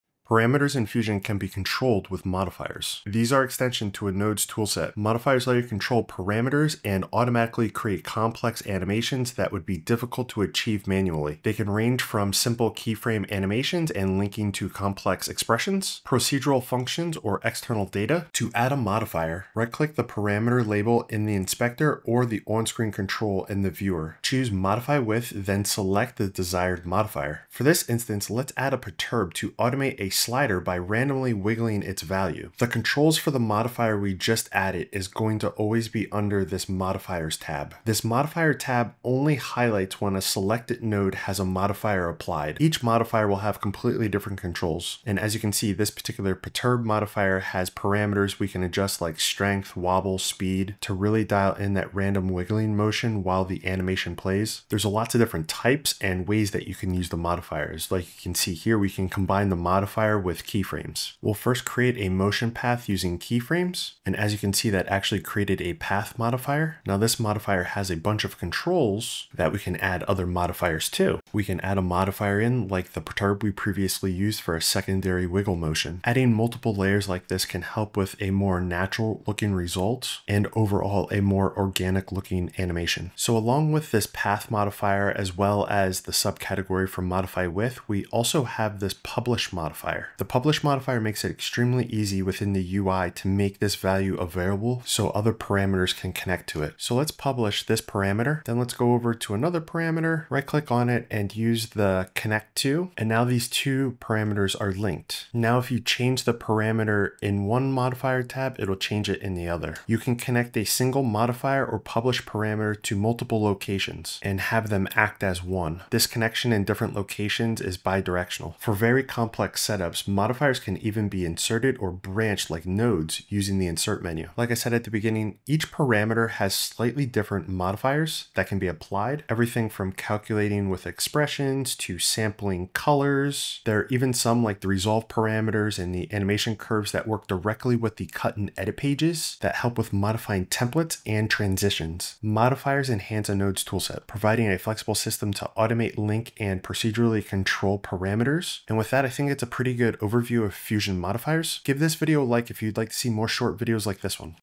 DaVinci Resolve Fusion Modifiers In 3 Minutes
Control & automate parameters in Fusion: link settings, keyframes, expressions, auto animation.

📒Topics Covered
* What are Fusion modifiers?
* How to add a modifier in DaVinci Resolve Fusion?
* Where to find Fusion modifier controls?
* How to combine Fusion modifiers and keyframes?
* How to link parameters in Fusion?
* How to use the Publish modifier in Fusion?
* What are different types of Fusion modifiers?
* How to automate animation in Fusion Resolve?